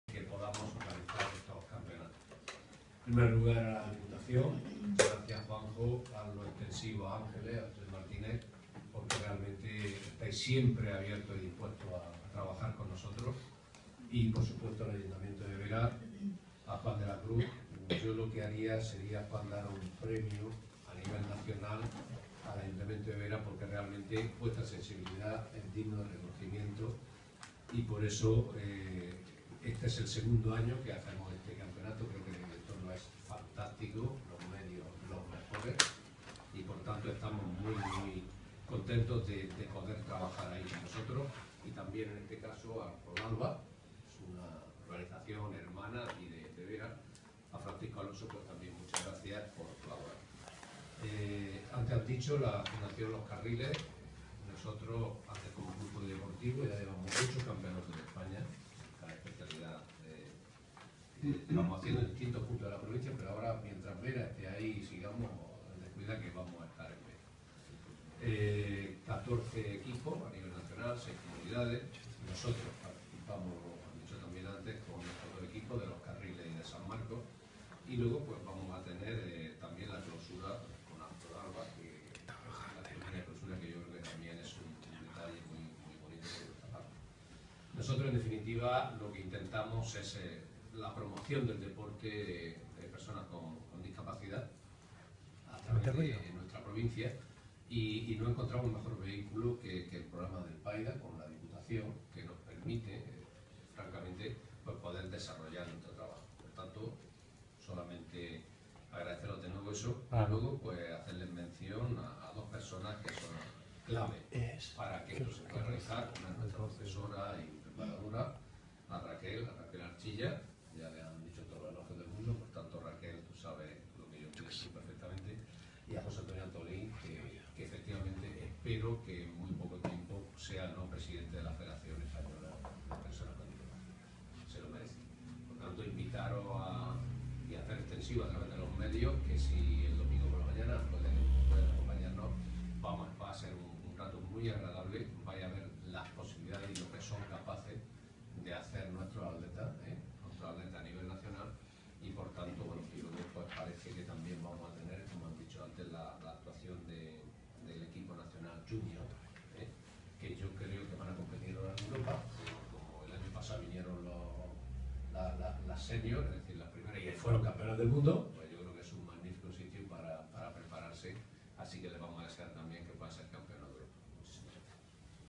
La Diputación apoya a Vera como sede del Campeonato de España de Gimnasia Rítmica (II)
El Pabellón Blas Infante de Vera acogerá por segundo año consecutivo el Campeonato de España de Gimnasia Rítmica de FEDDI que esta mañana se ha presentado de manera oficial en la Diputación Provincial en un acto en el que han participado el director de Deportes y Cultura de la institución, Juan José Salvador, concejal de Deportes de Vera, Juan de la Cruz Belmonte, presidente de Los Carriles, Diego Martínez Cano, delegada provincial de la Federación Andaluza de Gimnasia Rítmica, Raquel Archilla, vicepresidente de FEDDI, José Antonio Antolín y presidente de ASPRODALBA, Francisco Alonso.